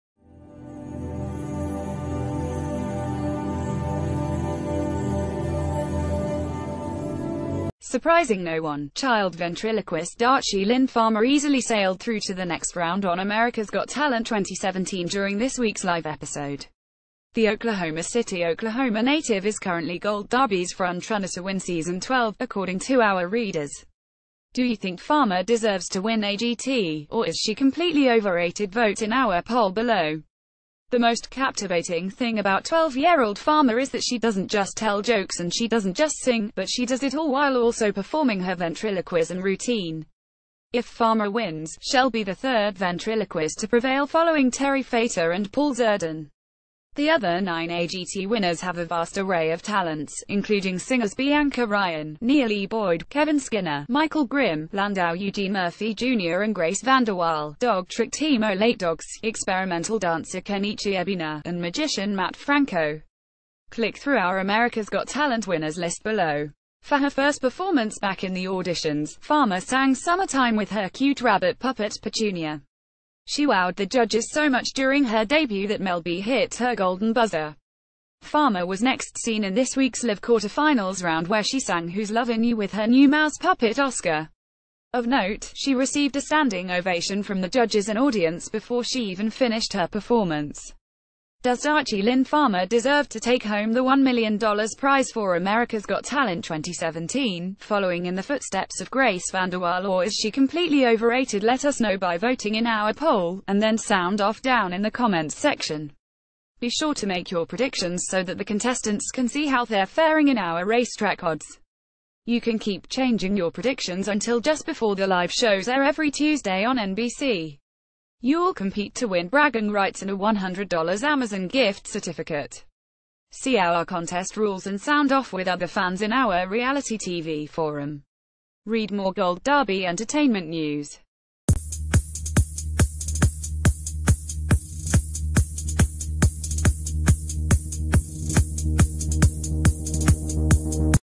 'america's got talent' 2017: does darci lynne farmer deserve to win 'agt,' or is she completely ove
'america's got talent' 2017: does darci lynne farmer deserve to win 'agt,' or is she completely overrated? [poll]